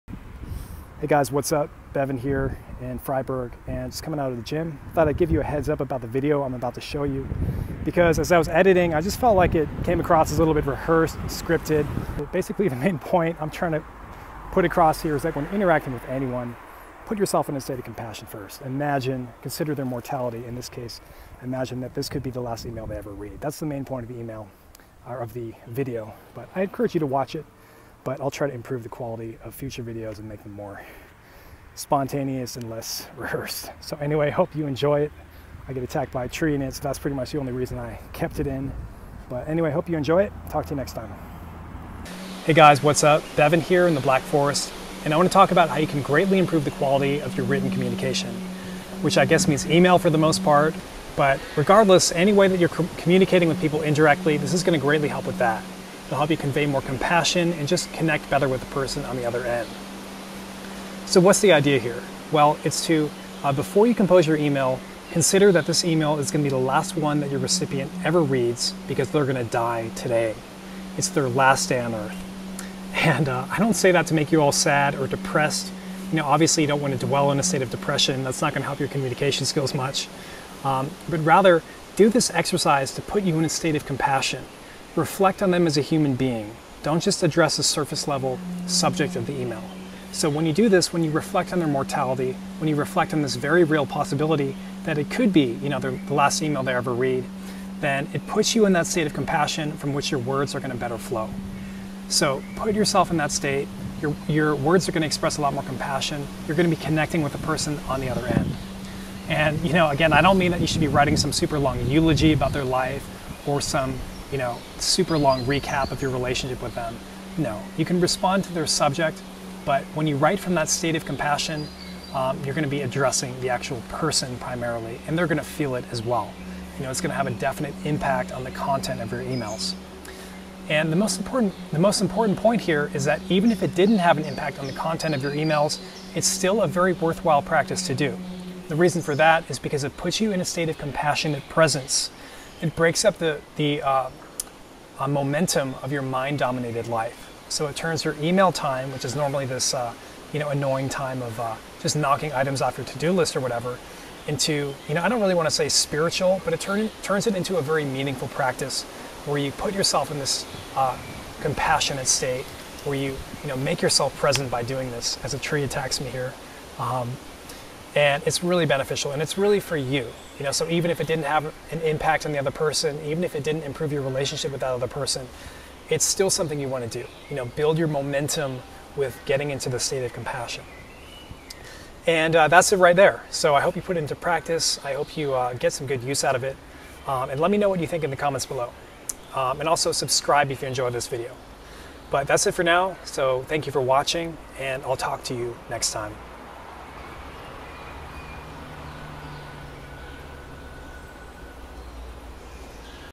Make Your Emails 100% Better By Contemplating This One Thing
Do this for its own sake (the act of putting yourself in a state of compassion) rather than for a specific reaction from your recipient.